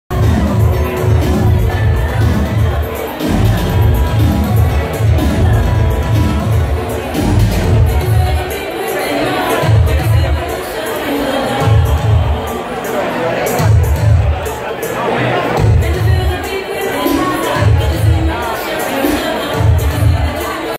Fiesta "Ay Not Dead" en Buenos Aires.
La reconocida marca festejó este jueves 24 de mayo, con una gran convocatoria en la noche porteña.